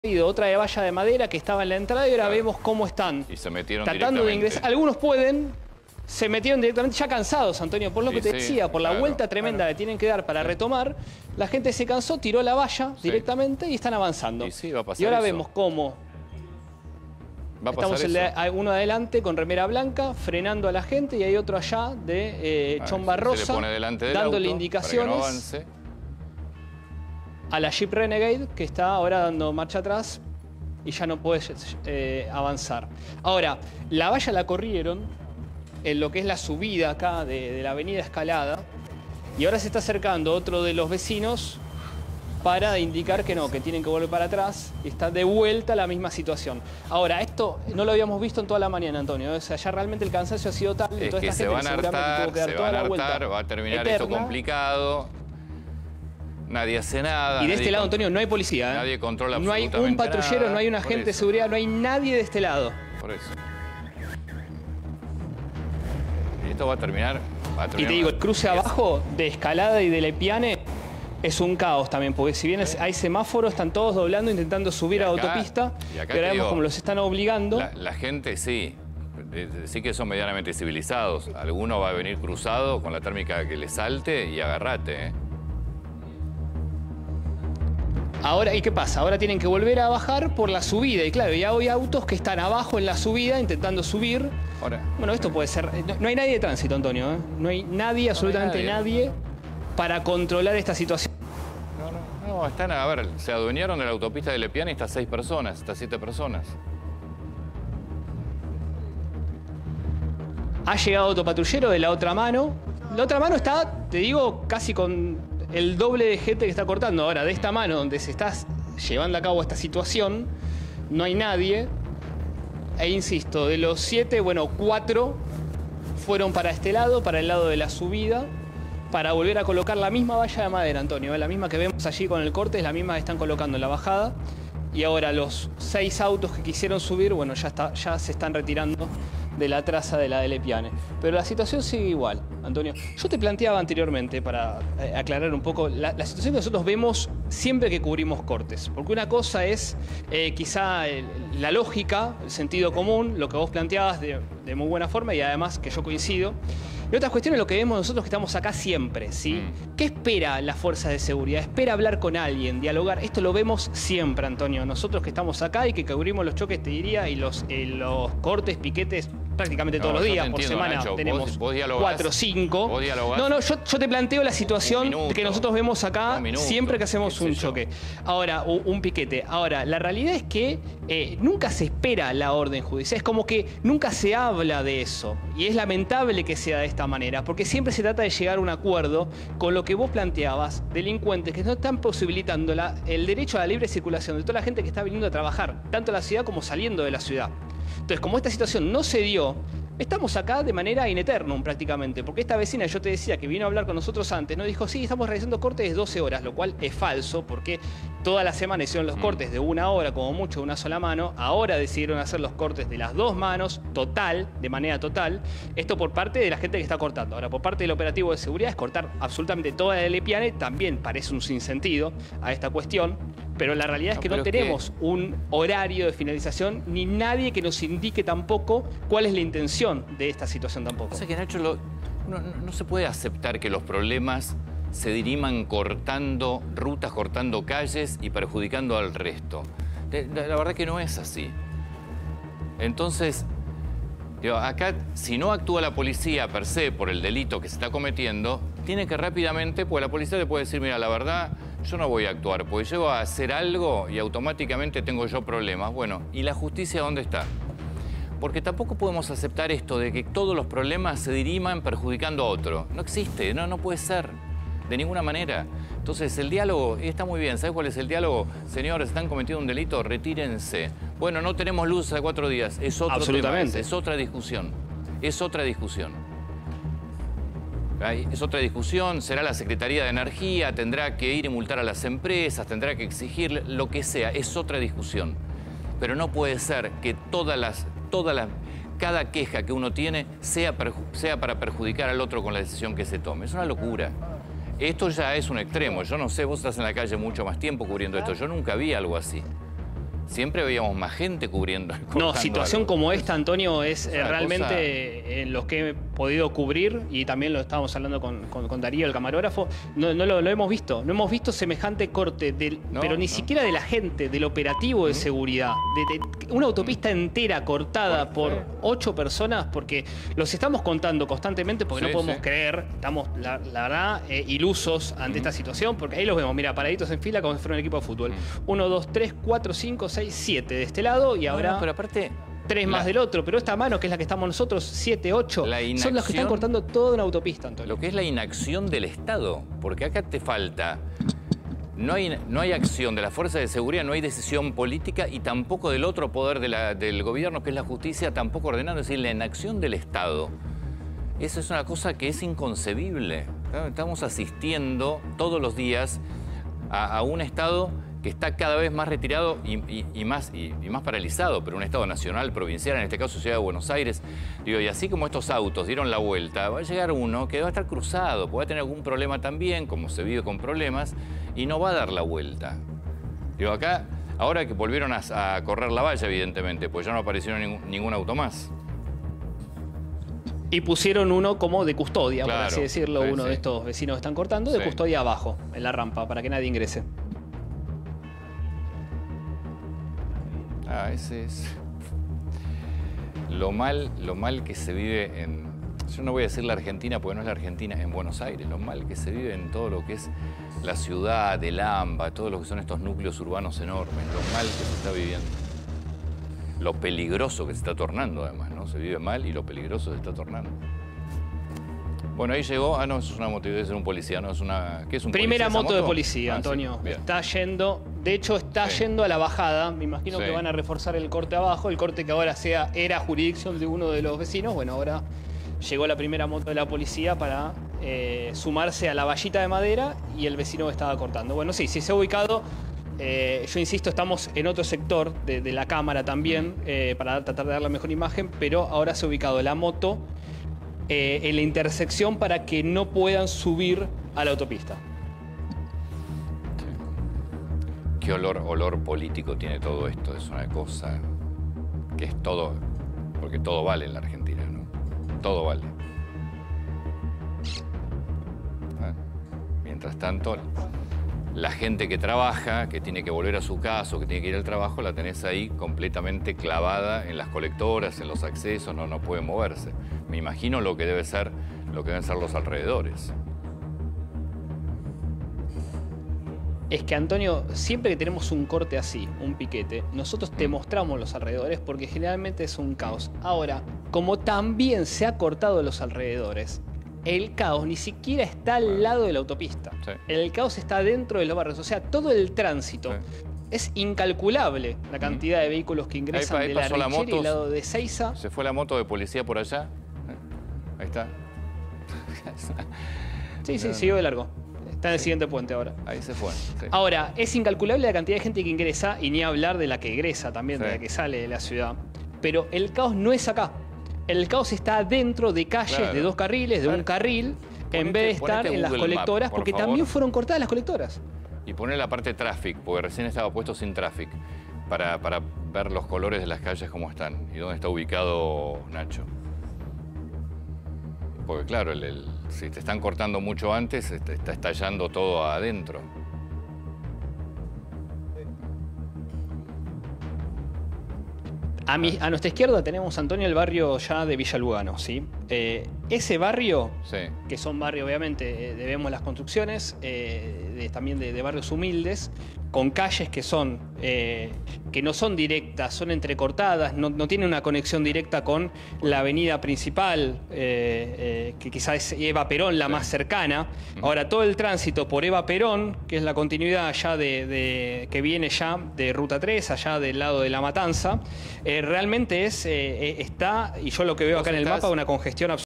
Vecinos cortan Au Dellepiane para reclamar por los cortes de luz
Móvil en vivo desde el lugar